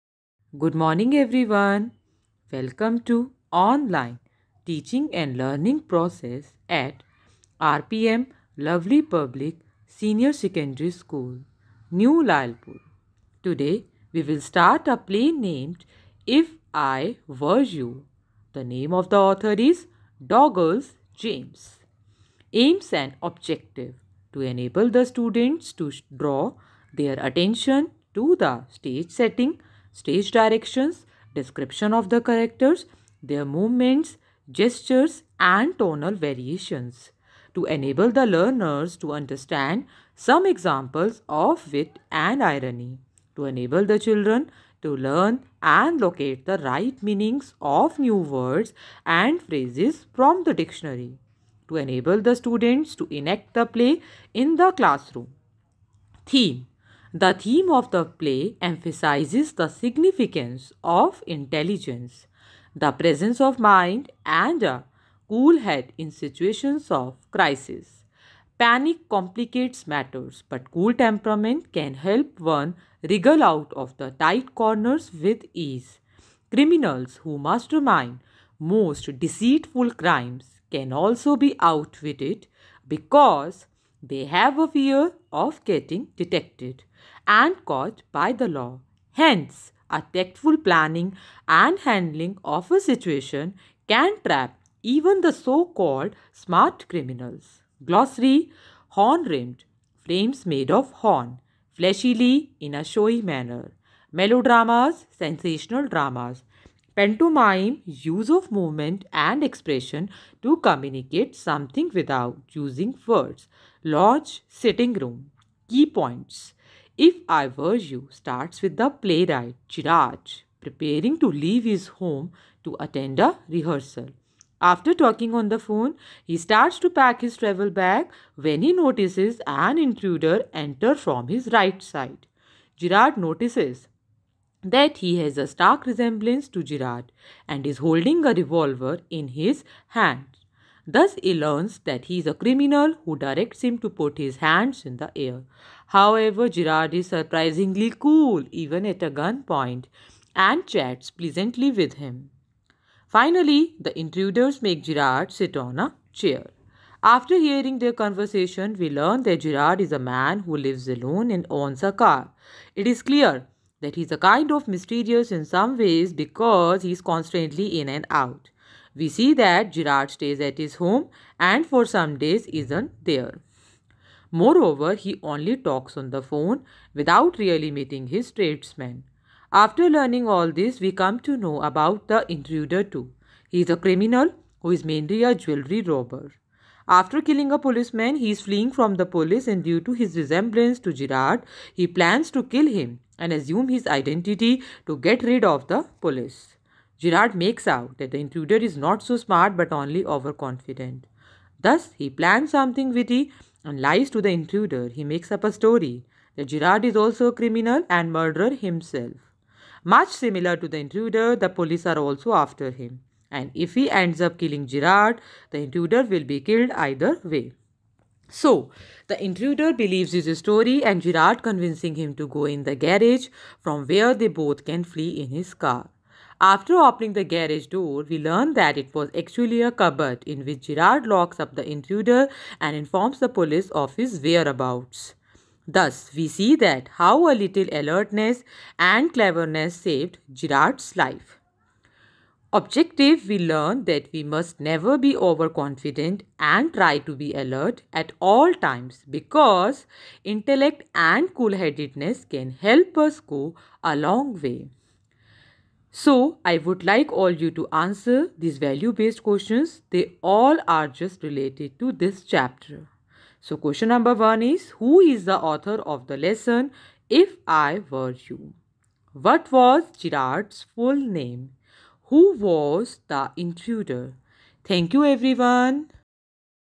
Class-IX Subject-English Chapter- If I were You
The video describes the key points of the chapter IF I WERE YOU and helps you to do a last minute revision of the chapter.

the beggar